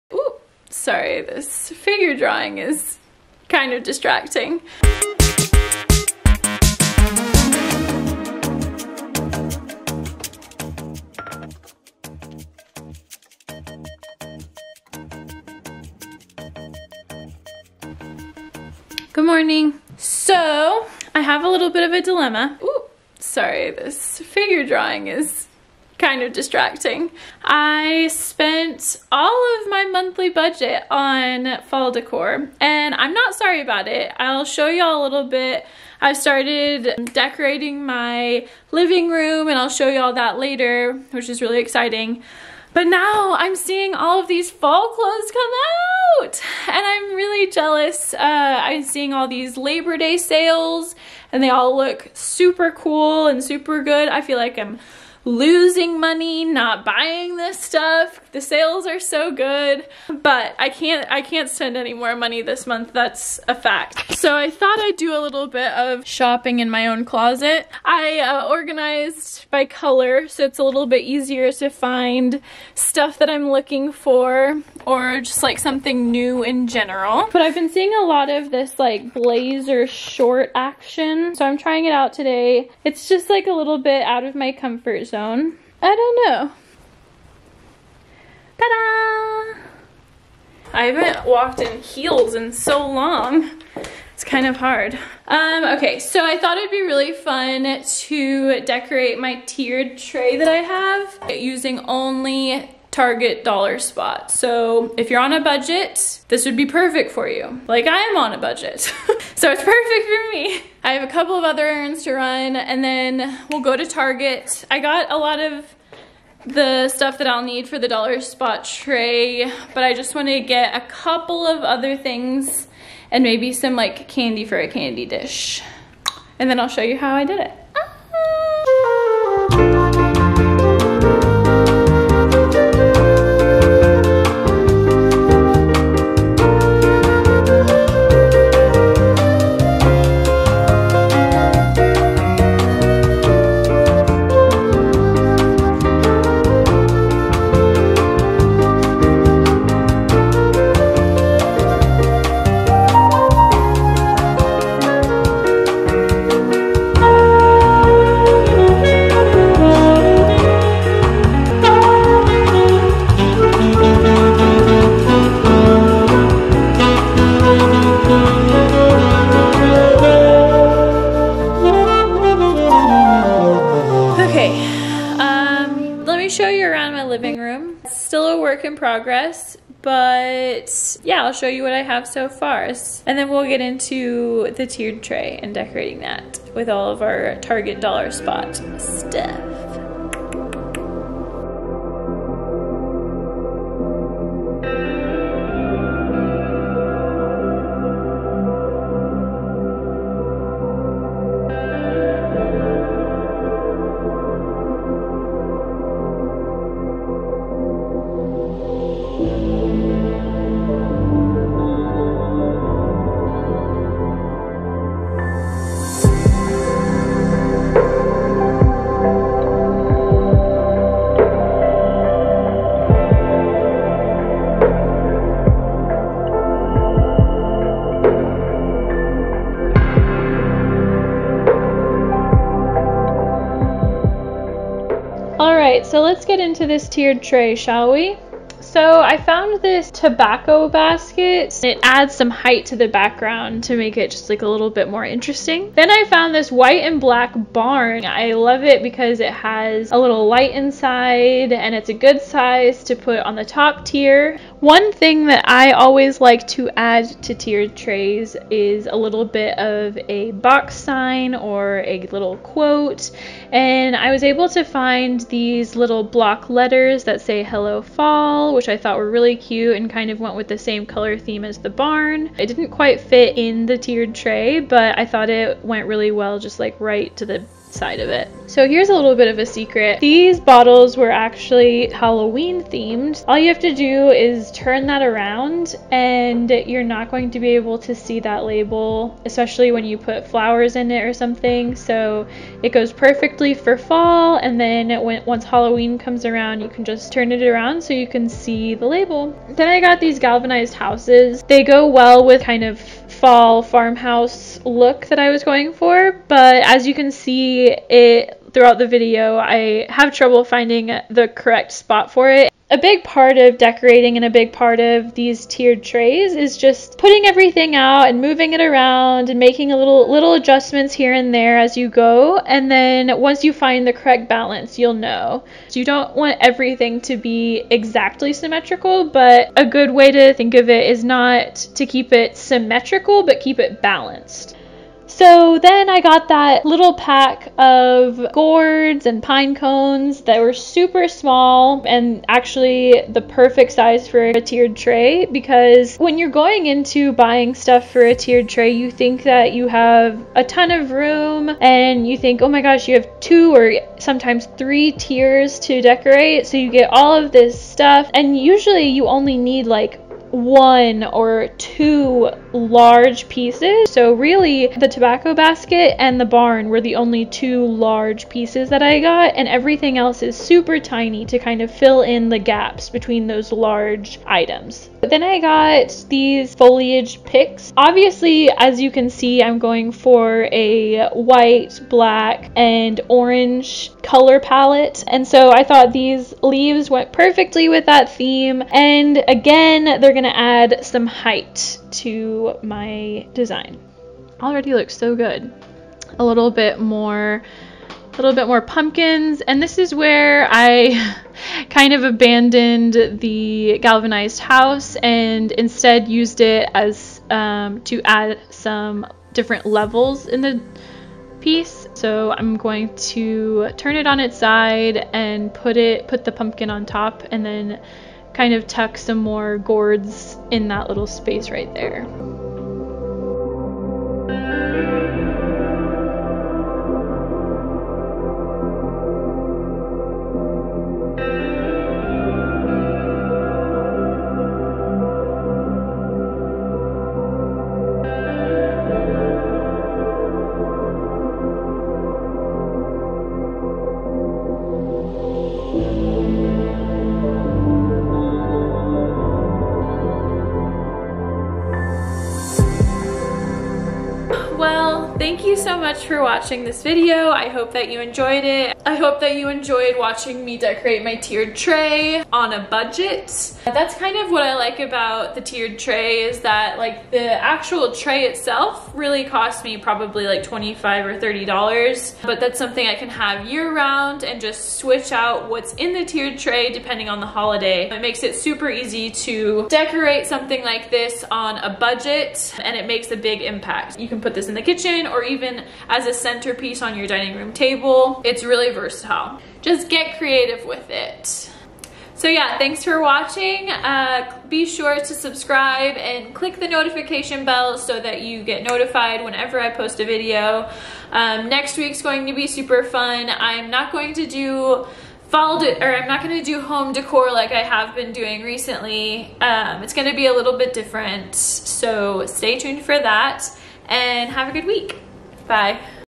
CHEAP TIERED TRAY STYLING FOR FALL | Target Dollar Spot Budget Friendly Decorate with me | VLOG 2020
I've spent ALL of this month's budget on fall decor, so let me show you how I style my tiered tray on a super tight budget using only items from Target Dollar Spot!